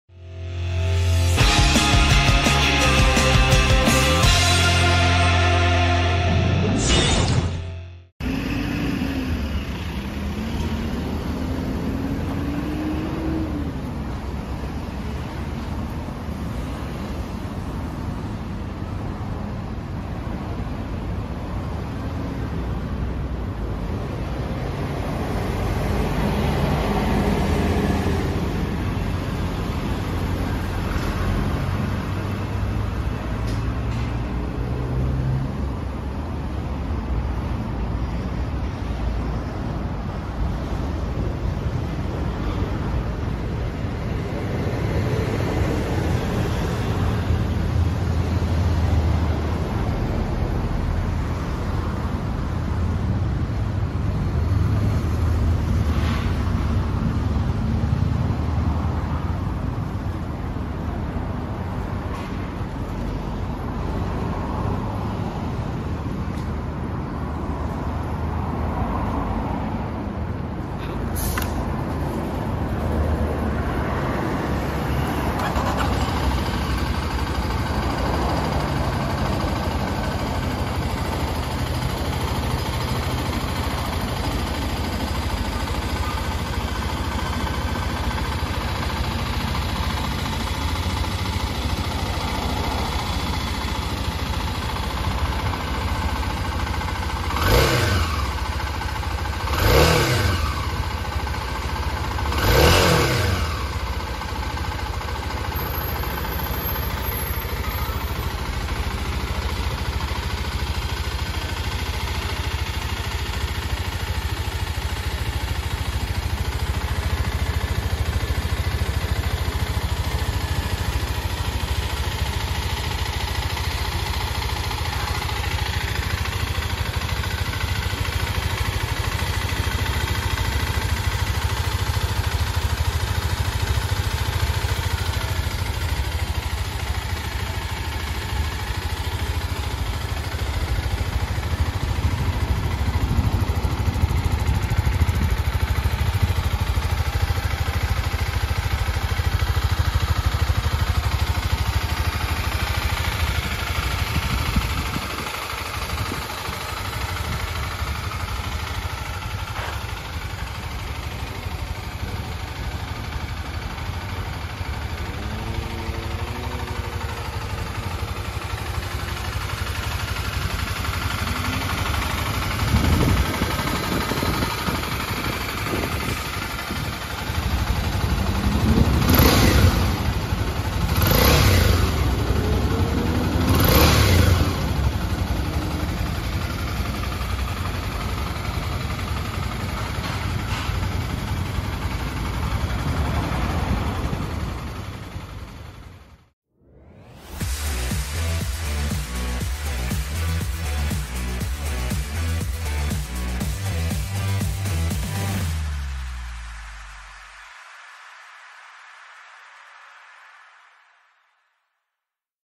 カワサキ Kawasaki ＫＬＸ２５０ エンジン音 純正リヤキャリアにＦＭＦパワーボム装着のファイナルモデル！キレイな１台です！
各部詳細画像はこちら！